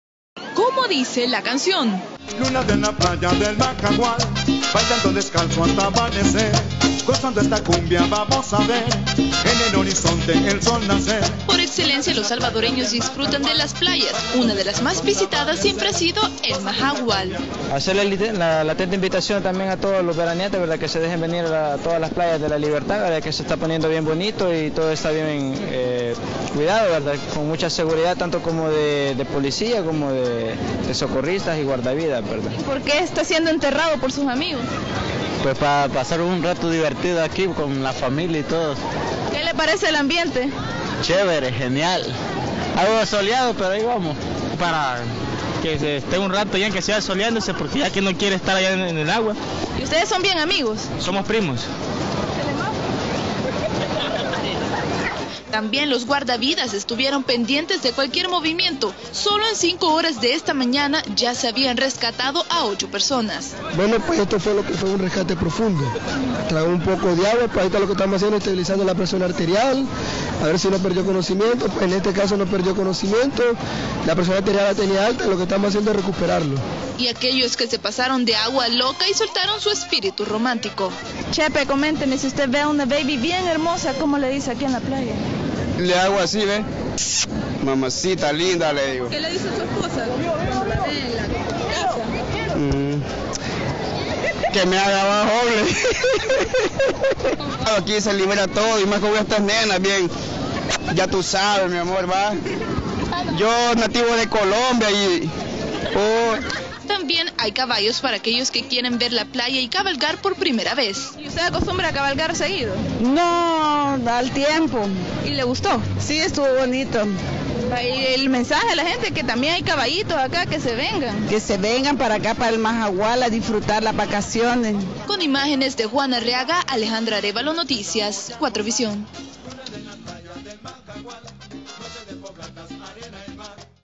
Majahual
Alejandra Arévalo nos presenta como disfrutaron muchos salvadoreños en estas vacaciones de Semana Santa en una de las playas más frecuentadas en La Libertad.